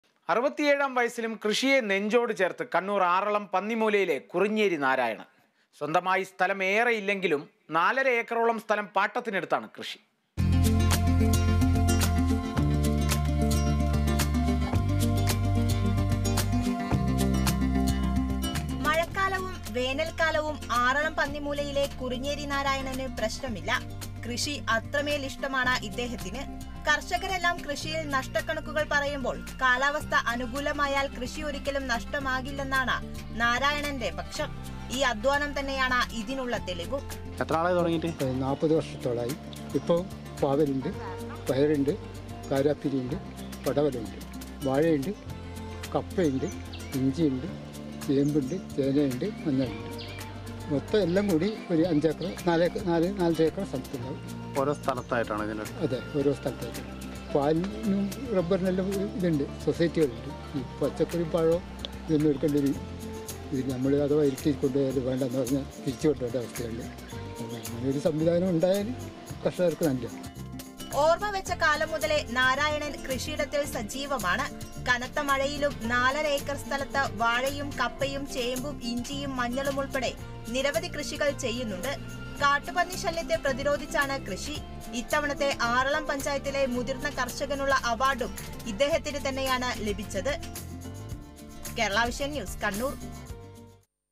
67ാം വയസ്സിലും കൃഷിയെ നെഞ്ചോട് ചേര്‍ത്ത് കണ്ണൂര്‍ ആറളം പന്നിമൂലയിലെ കുറിഞ്ഞേരി നാരായണന്‍
67ാം വയസ്സിലും കൃഷിയെ നെഞ്ചോട് ചേര്‍ത്ത് കണ്ണൂര്‍ ആറളം പന്നിമൂലയിലെ കുറിഞ്ഞേരി നാരായണന്‍. സ്വന്തമായി സ്ഥലം ഏറെ ഇല്ലെങ്കിലും നാലര ഏക്കറോളം സ്ഥലം പാട്ടത്തിനെടുത്താണ് കൃഷി.

About COA